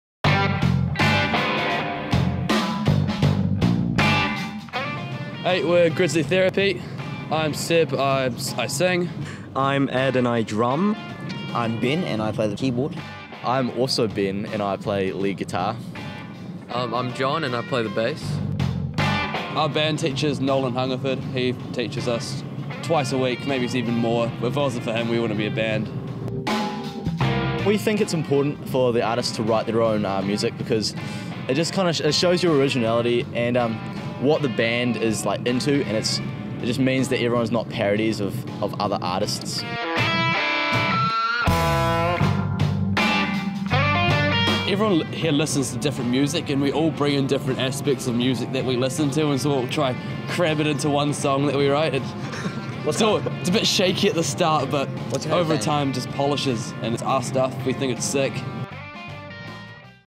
Grizzly Therapy - Artist Profile Series 2018
Smokefreerockquest Artist Profile Series
Episode 30 of 100
Grizzly Therapy

Grizzly Therapy are a Christchurch based band from Christ's College.